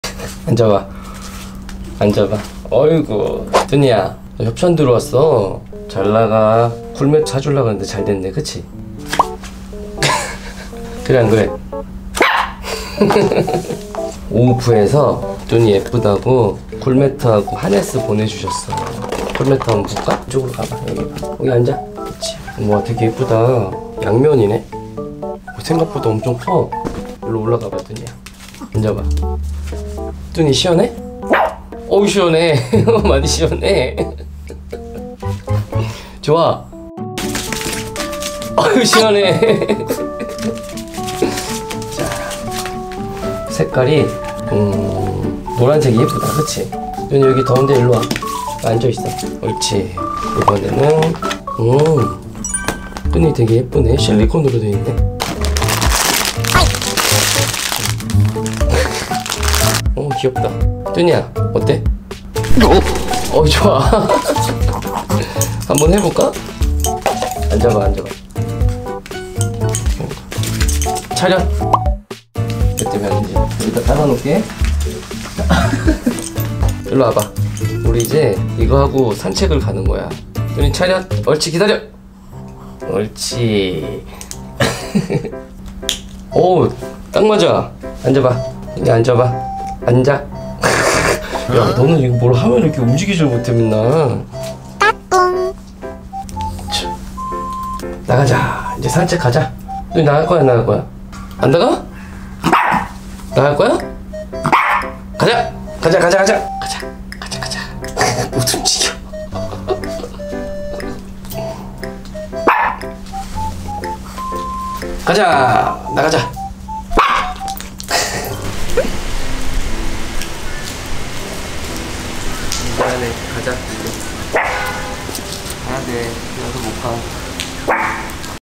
협찬 강아지 쿨매트 생겨서 개좋아
이번에 오우프 에서 예쁜 쿨매트와 하네스를 협찬해주셔서 영상을 만들어봤어요 쿨매트가 너무 예쁘고 생각보다커서 좋네요 하네스는 실리콘 제질로 되있어서 흰색은 금방 드러워 질가바 걱정했는데 물티슈로 닦으면 금방 지워 지네요  뚜니도 너무 좋아 하네요 ㅎㅎ
감사 합니다 오우프
안녕하세요 김뚜니 입니다

  틱톡

이름 - 김뚜니
성별 - 여